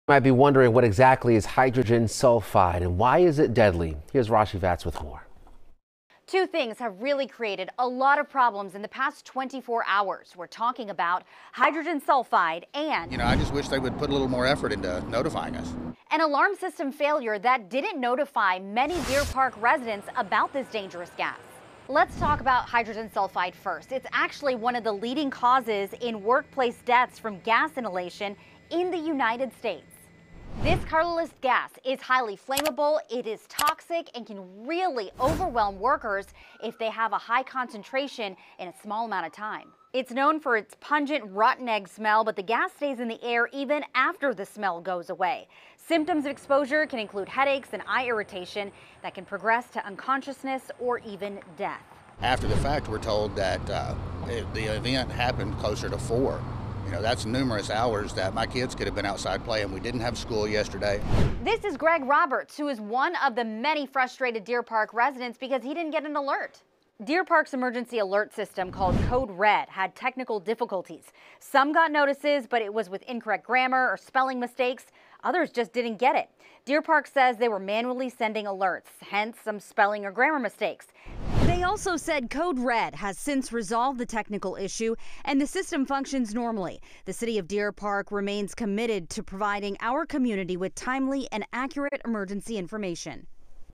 What is hydrogen sulfide?
What is the potentially deadly gas that was realeased at the Deer Park Pemex facility.